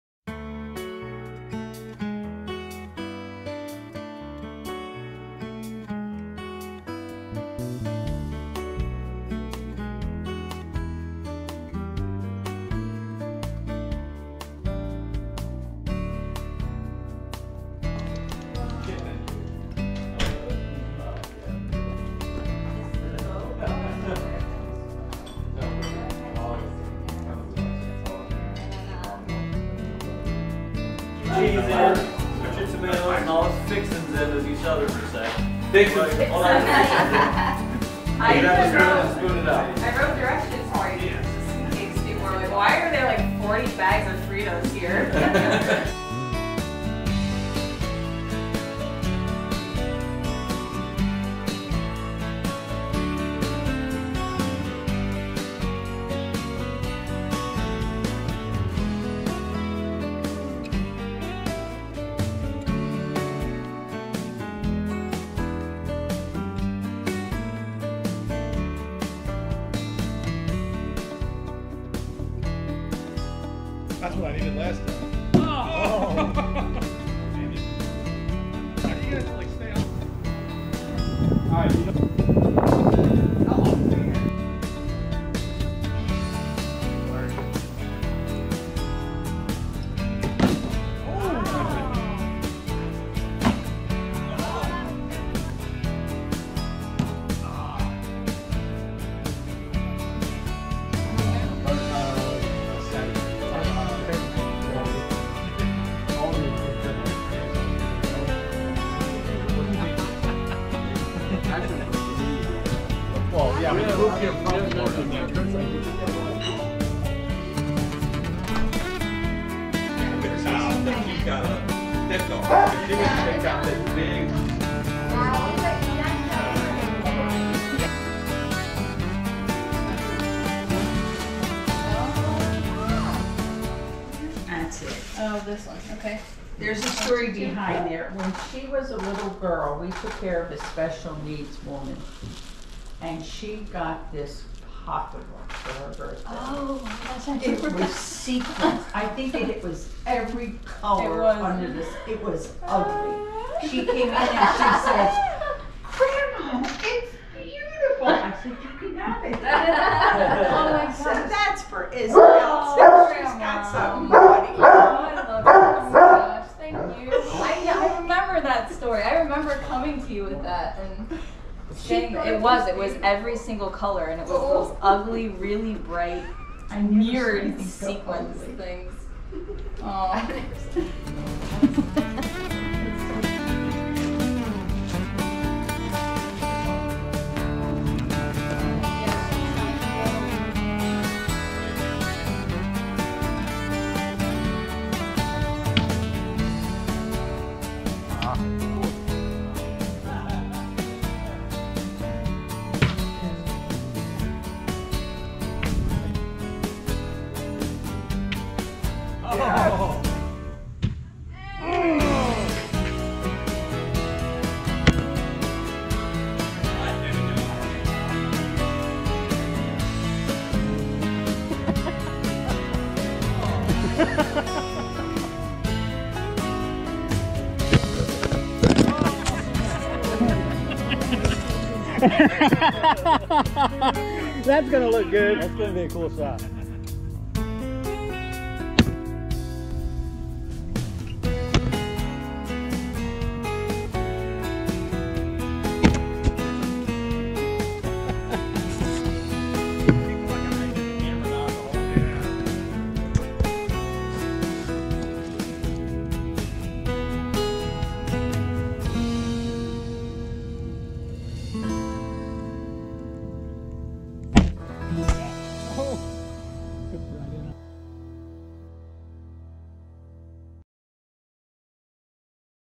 Celebrating Baby Isabella
A quick video of the Digati baby shower for Isabella! We are all so excited to meet her and watch the Digati family expand. Such a wonderful afternoon!